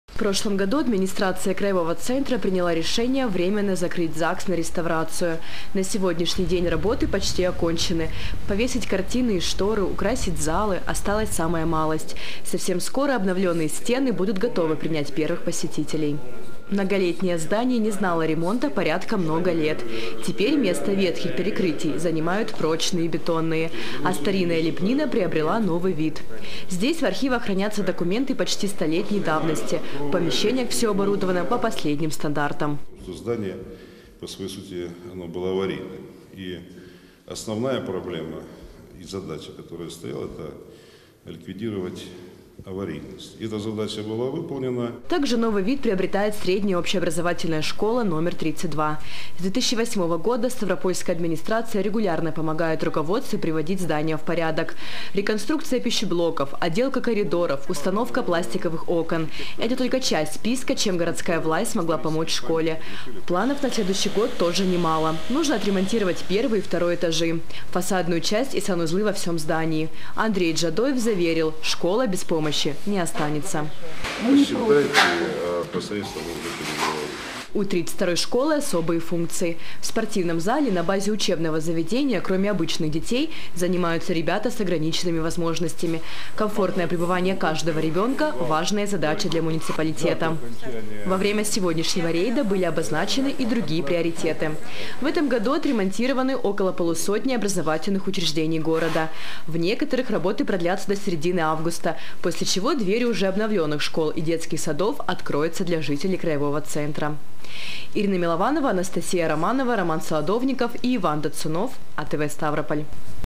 Ремонт в городе
В этом году ремонту подверглось порядка полусотни городских учреждений.  В некоторых работы продлятся до середины августа, а затем двери уже обновленных городских учреждений откроются для жителей краевого центра.